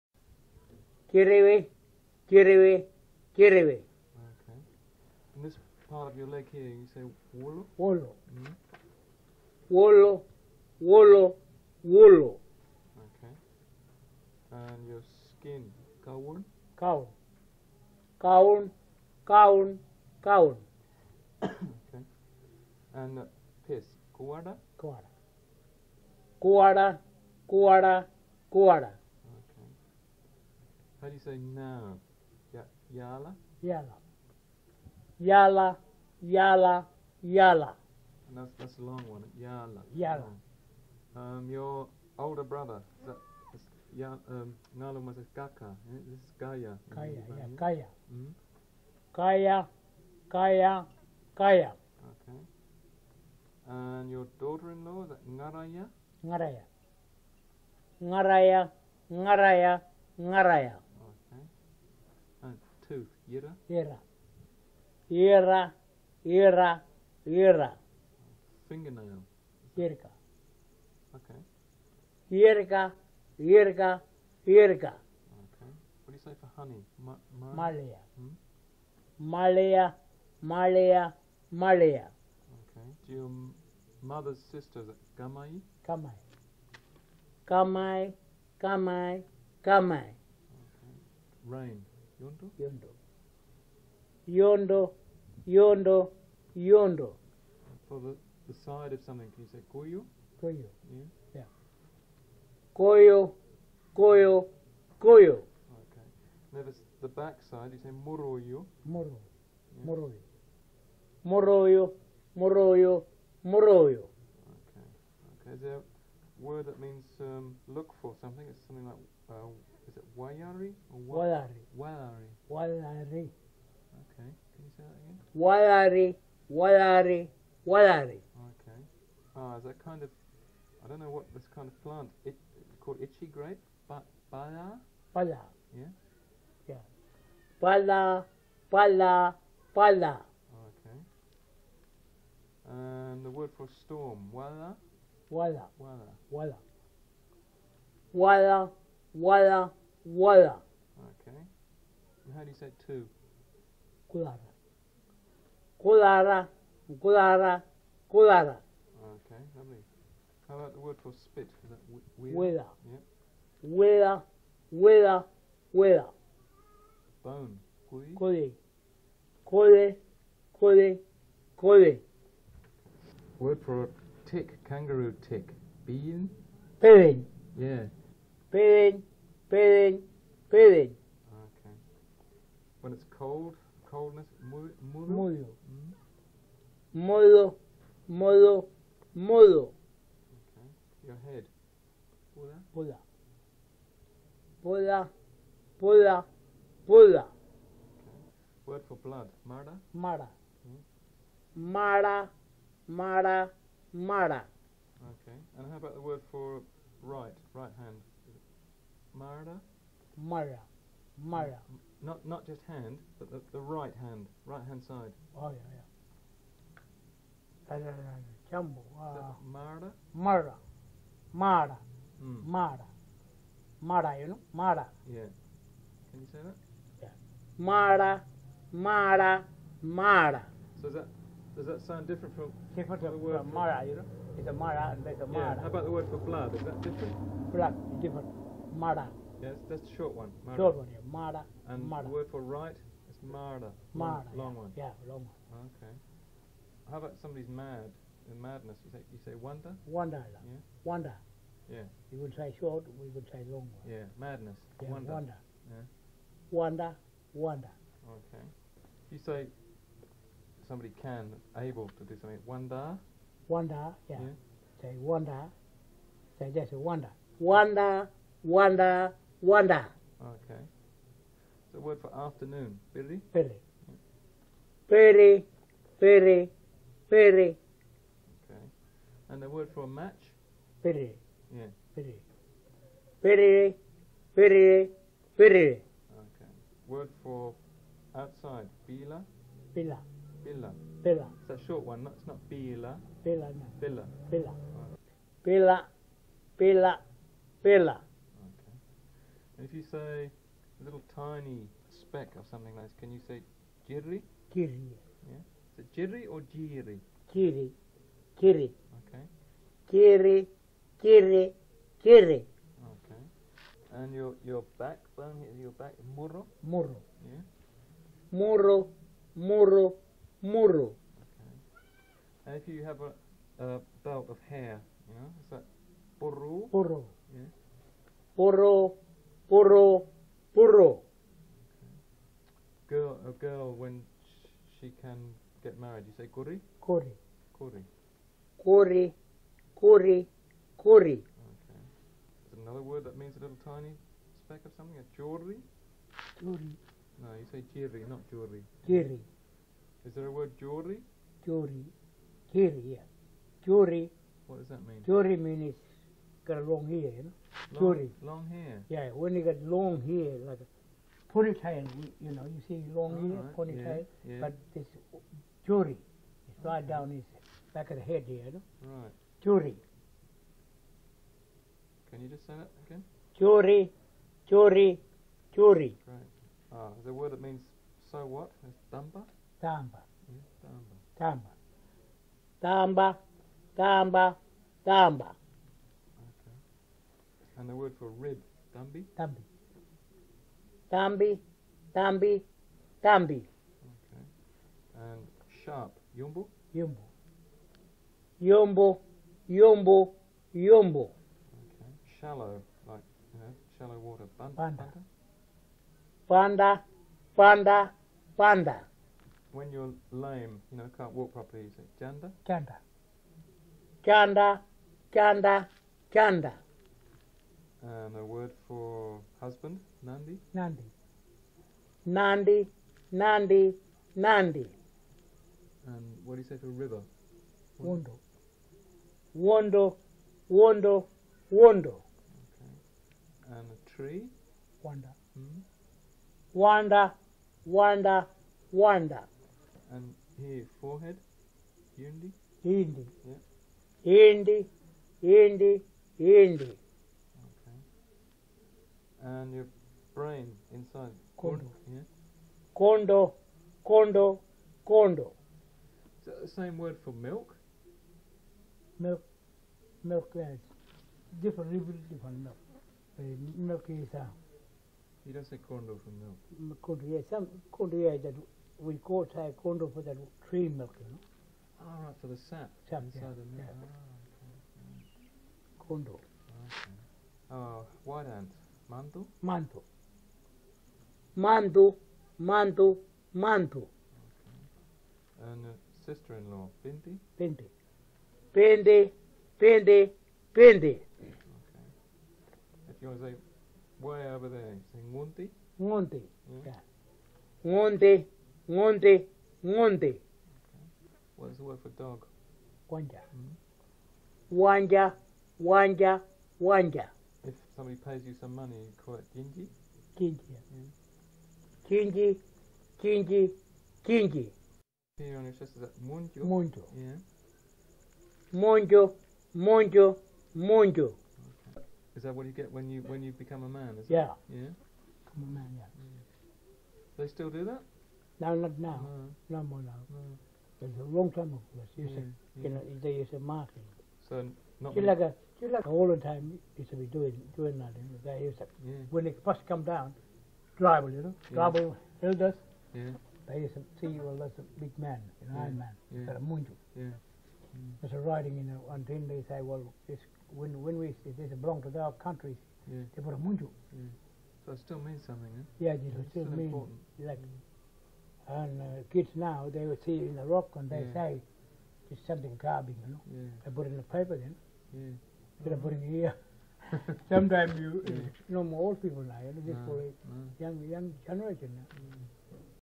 Yindjibarndi audio: yij_word-list_1990_04
Yindjibarndi (yij) audio: yij_word-list_1990_04
Language family: Australian; Pama-Nyungan; South-West Pama-Nyungan; Coastal Ngayarda;
Roebourne, Woolshed, Western Australia
Date: 3/27/1990

Description: Language Yindjibarndi.  yij_word-list_1990_04.html, enties: 1 - 50

Contributor: Yilbie Warrie
UCLA Phonetics Archive